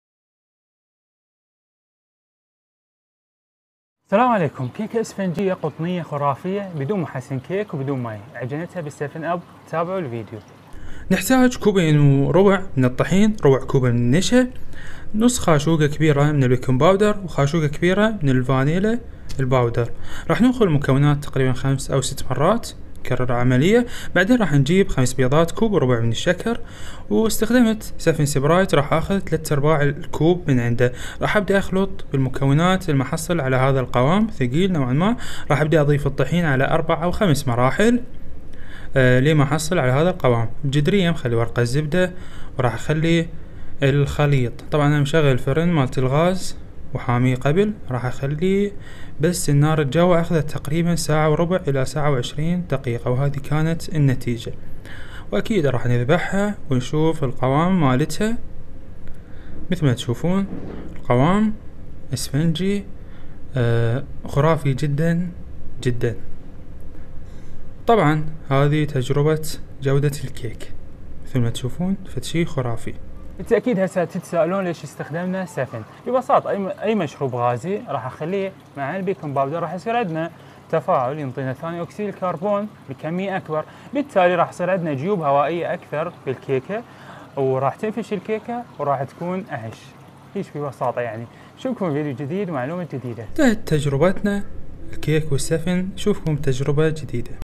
كيك اسفنجي ممتاز بأستخدام السفن اب ،بدون محسن كيك
كيكة اسفنجية خرافية باستخدام السفن اب😁😁 بدون محسن كيك .

المكونات
٢وربع كوب طحين
ربع كوب نشأ
٥ بيضات
خاشوكة كبيرة فانيلا باودر
كوب وربع شكر
تلث ارباع الكوب سفن
نص م كبيرة بيكنغ باودر
خاشوكة كبير خل ابيض
نكهة الهيل